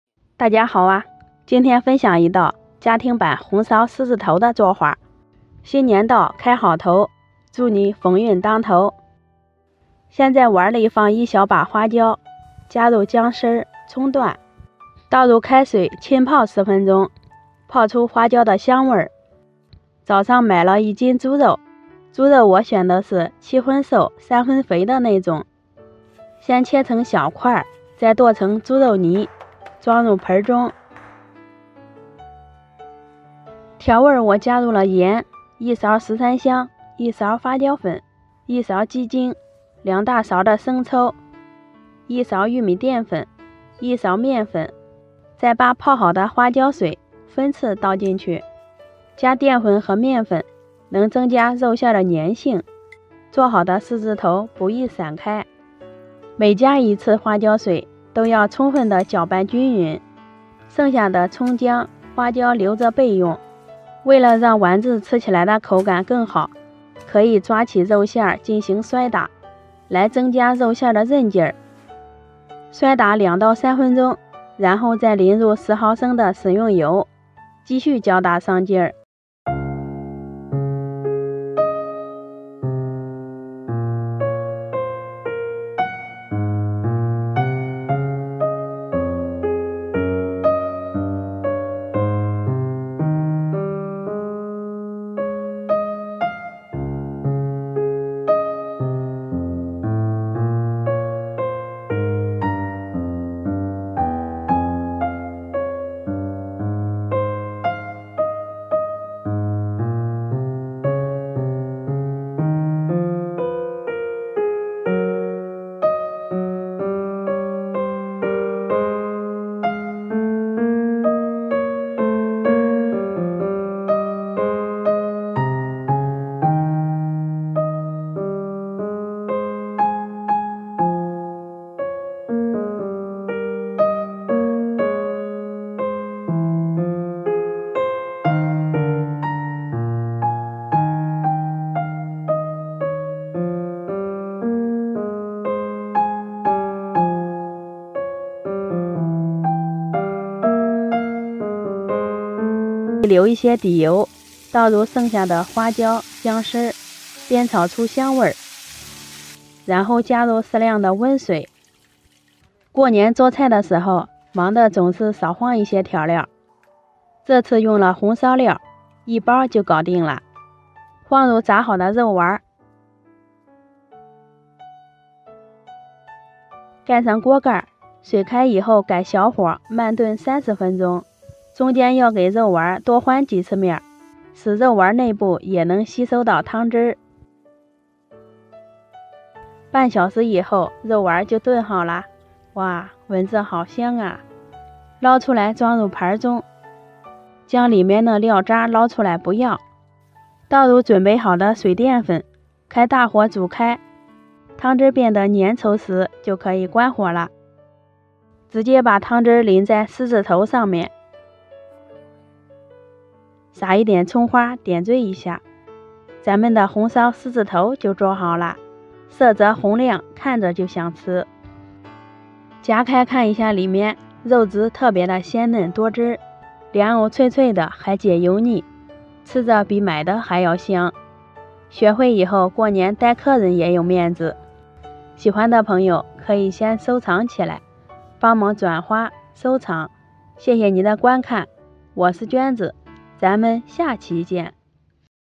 給年夜飯加道菜“紅燒獅子頭”端上桌，家庭版做法，寓意鴻運當頭【娟子美食】
紅燒獅子頭是一道淮揚名菜，出鍋後撲鼻的香味，光聞起來就會引動食慾，醇香味濃的肉丸超級美味。而且營養也比較豐富，口感滑嫩，老少皆宜。過年用來招待客人有面子！
娟子在視頻中為大家添加了英語，越南語，西班牙語，印地語，大家可以點擊屏幕右下角，設置字幕哦！